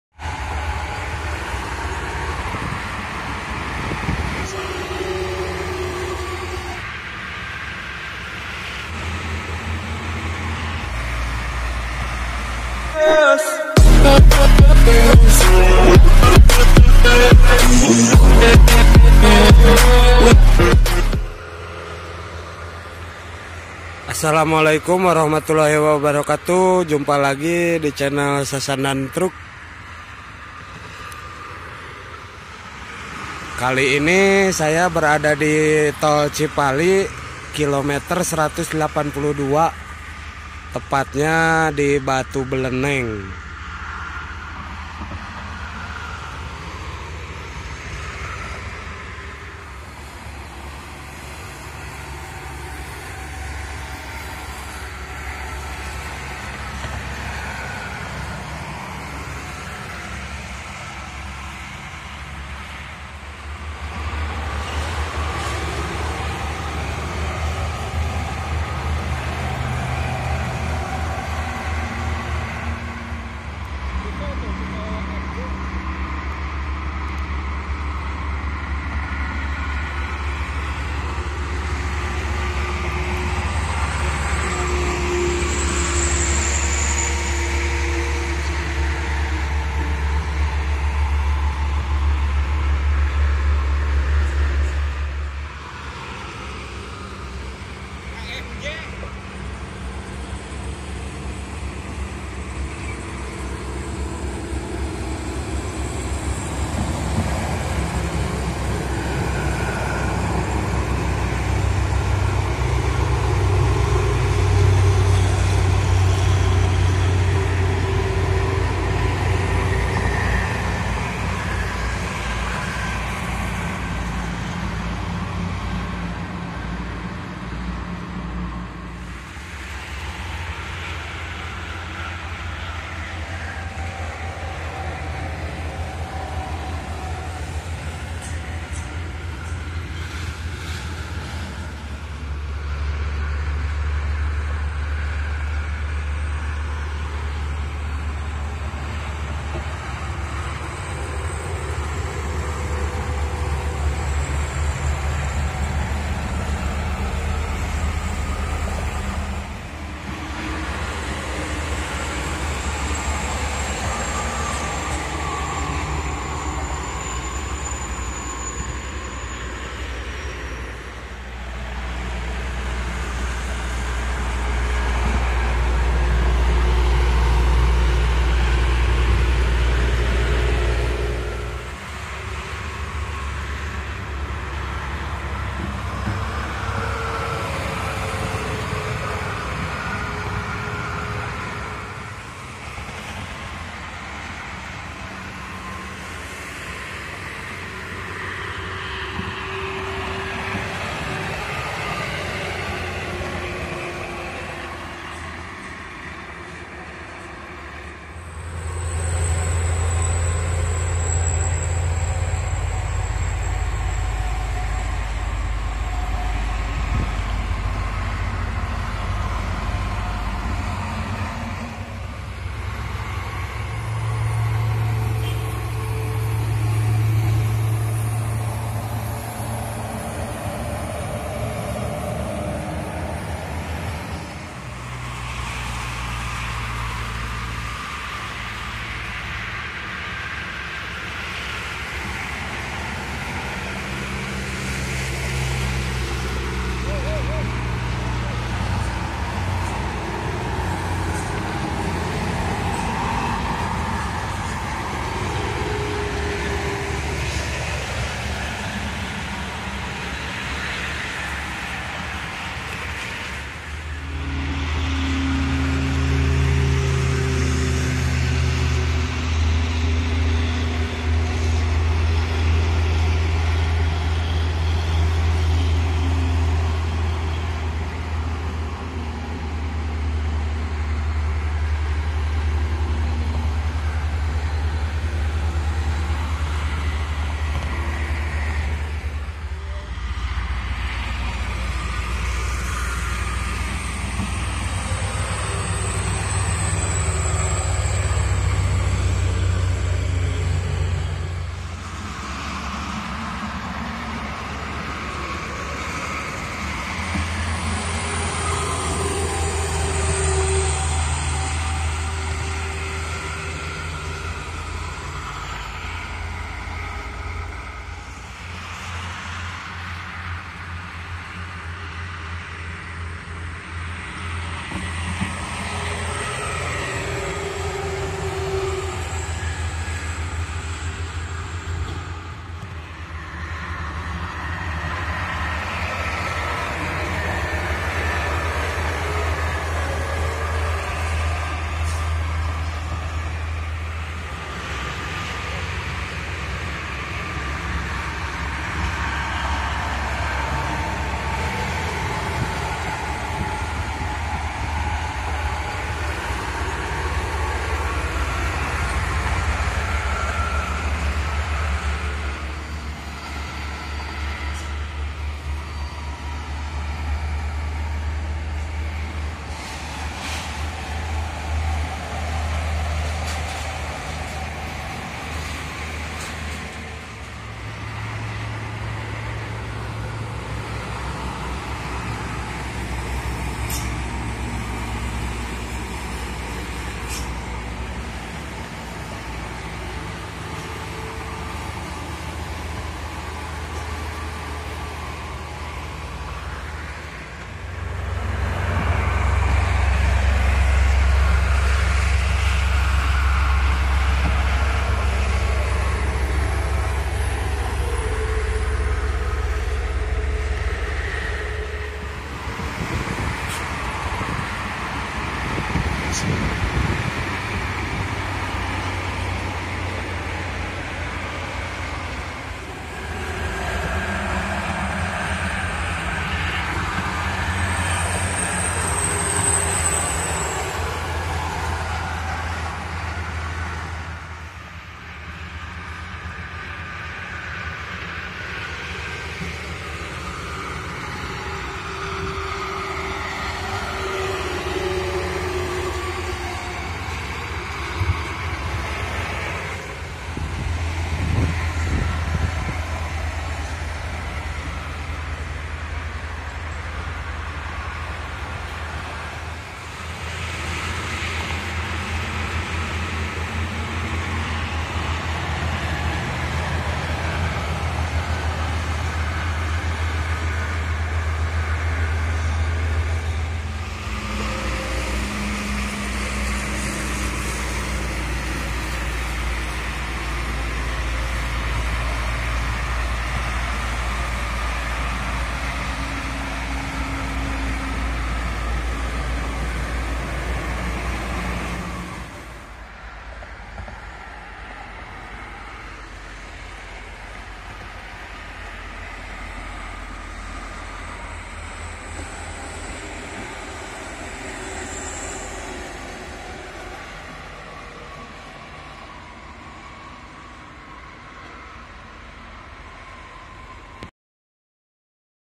Mantau Pelari 🌶️ Di Tol Trans Java Km 182
Truck-truck Modif Mbois Juga Terpantau Di Jalur Tol Trans Java Km182 Bahkan Terdengar Jelas Jeritan Turbo Ketika Truck Berlari Di Tol Lebih Serunya Check Di Vidio Jangan Di Skip